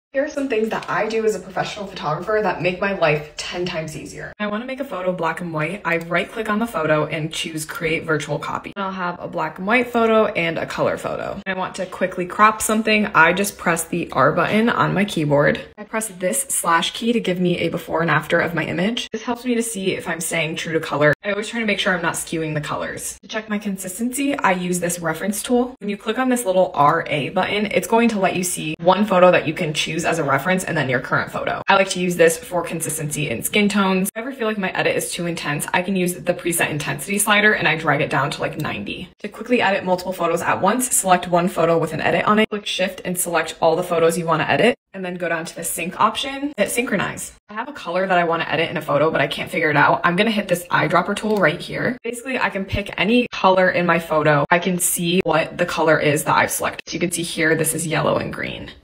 Lightroom Hacks | Photography Tips by Cassidy Lynne
Here are some things that I do as a professional photographer that make my life 10x easier. So here are my Lightroom Hacks!
- When I want to make a photo black and white, I right click on the photo and choose Create Virtual Copy. Then I'll have both black and white and a color photo.
- When I want to quickly crop something I just press the R button on my keyboard.
- Press the slash key on your keyboard to give you a before and after of your image.
- To check your consistency, use the reference tool. When you click on the RA button it's going to let you see one photo that you can choose as a reference and then your current photo.
- If I ever feel like my edit is too intense, I can use the preset intensity slider and drag it down to like 90%.
- To quickly edit multiple photos at once, select one photo with an edit, click shift and select all the photos you want to edit and then go to the sync option and just hit synchronize.
- If there's a specific color you want to edit, hit the eyedropper tool and pick any color in your photo and as you hold that down you can see what color it is that you've selected.